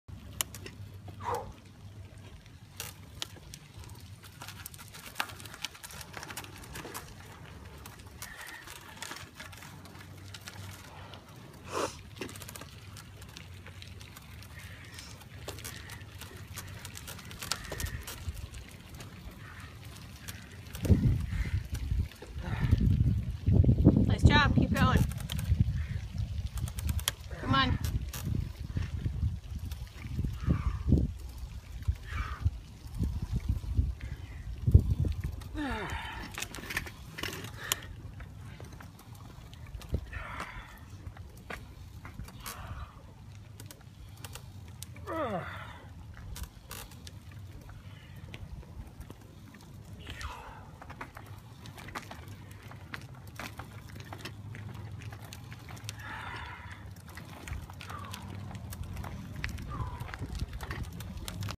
Quad Elite RX 2015 04 26 spring loop
C5-6 Quad in Bike-on Quad Elite RX with Baron controls ProShift auto shift system..
This is a loop I like to do by our house. the hill isn't huge so what I do is grind it in a bigger gears than normal, each climb I drop one gear until im smoked.  This was lap 5. Great way to get a nice burn.